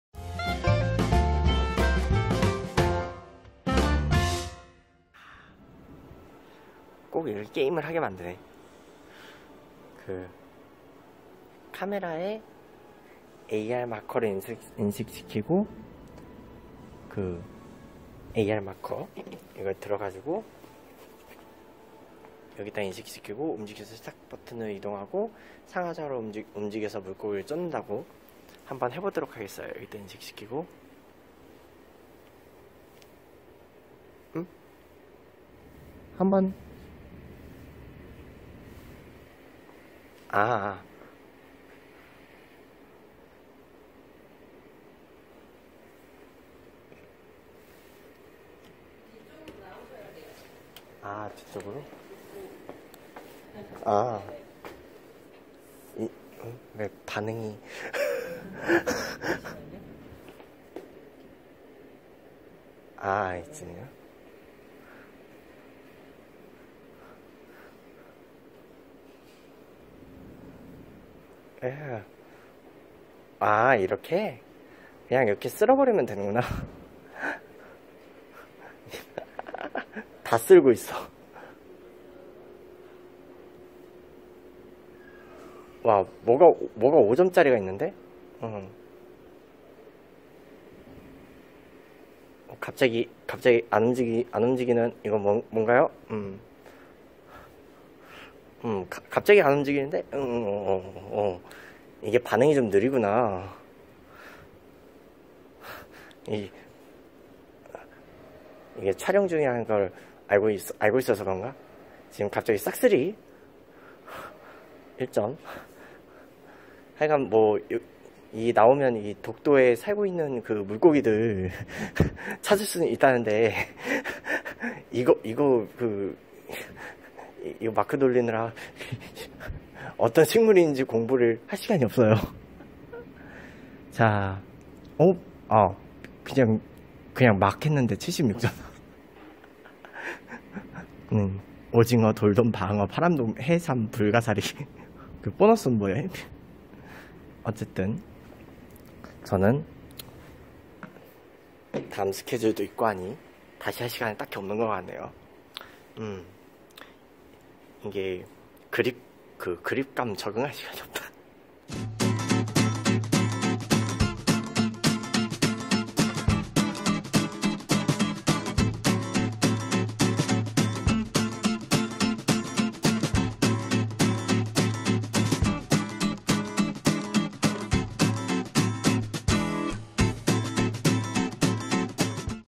독도 체험관에서 낚시를 해 봤어요 Dokdo | 지테TV 사심 史心 역사 112화 2부
저 정도 점수가 얼마나 잘 하는 축인지는 잘 모르겠어요. ;;